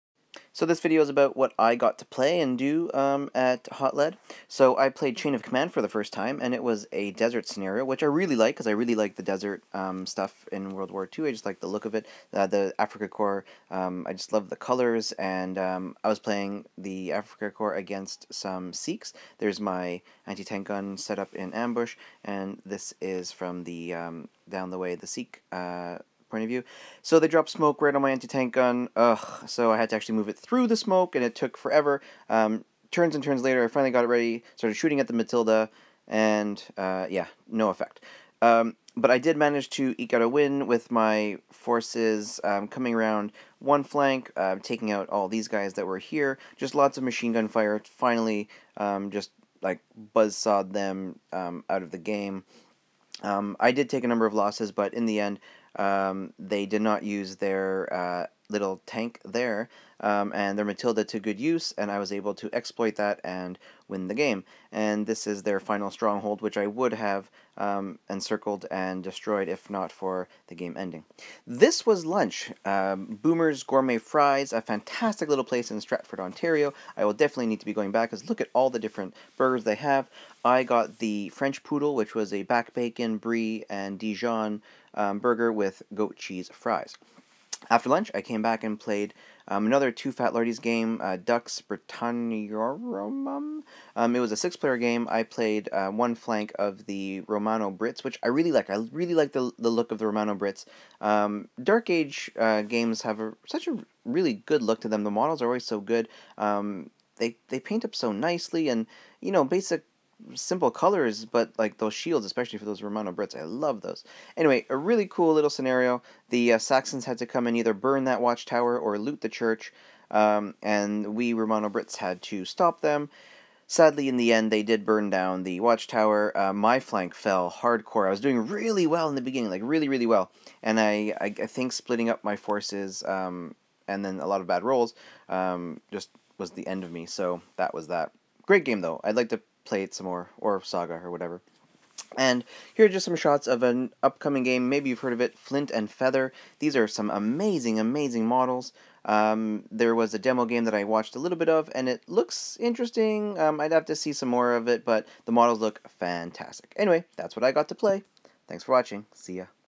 Hot Lead 2016 - Part 2
The games I got to play at Hot Lead 2016 in Stratford, Ontario.